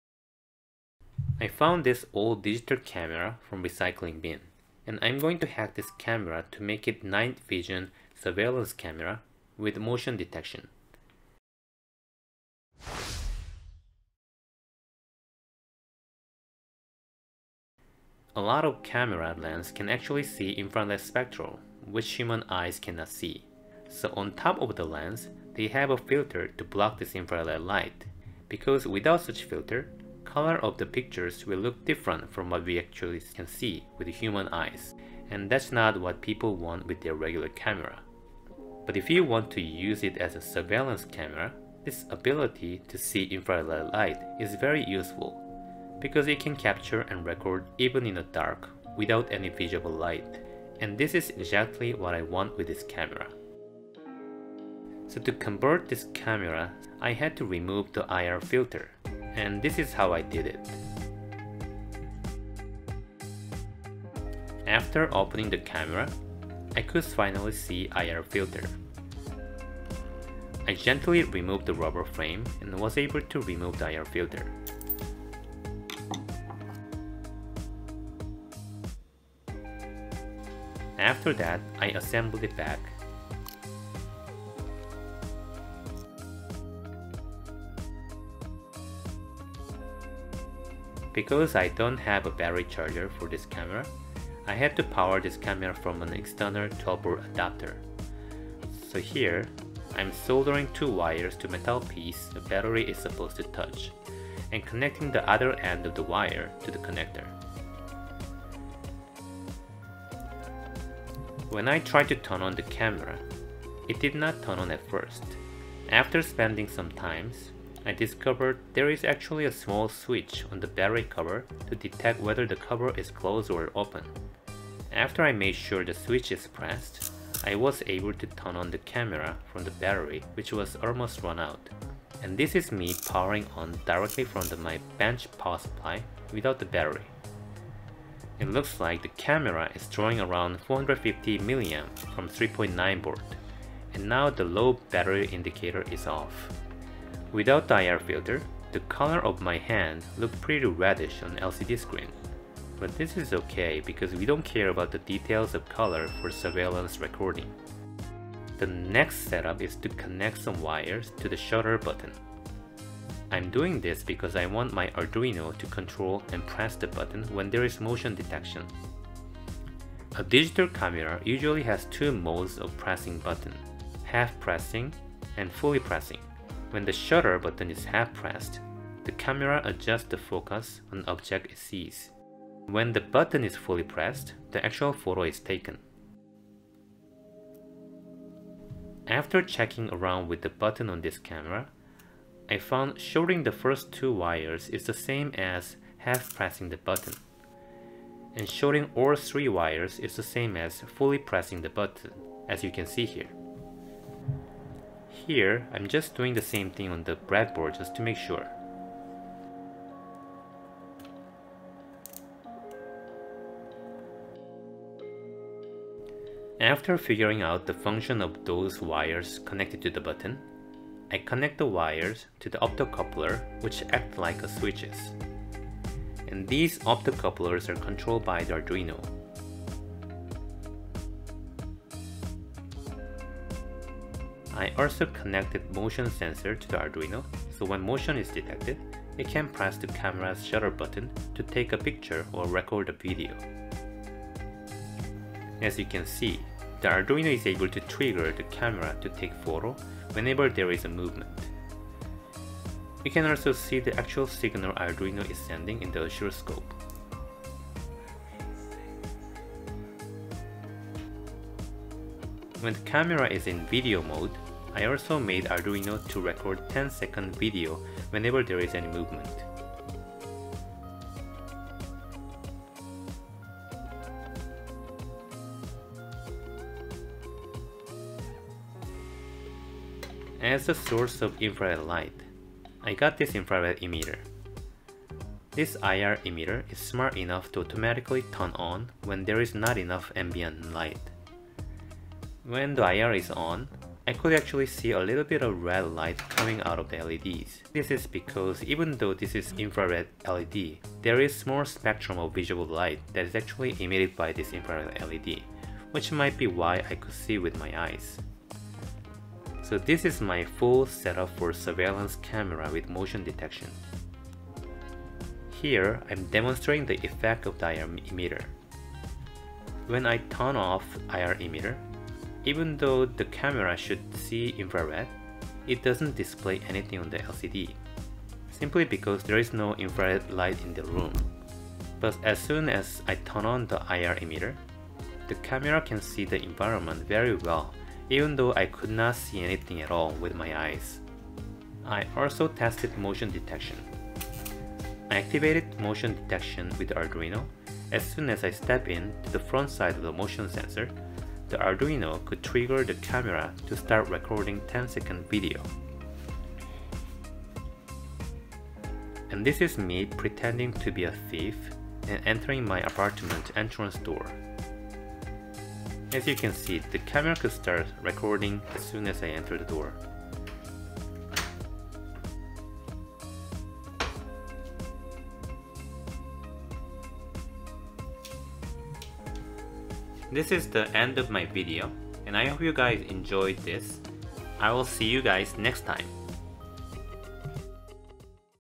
DIY Camera Hack - Transform old camera to Night Vision Surveillance Camera with Motion Detection
In this video, I am hacking an old digital camera someone threw away to convert to night vision surveillance camera. This can be done by removing the Infrared filter between the image sensor and the camera lens. I also connected the camera's shutter button to Arduino so that it can control the camera when motion is detected.

- Model of Camera: Cannon PC1235
- CCTV Infrared Light
- Arduino (similar model as one used in Video)

Curiousor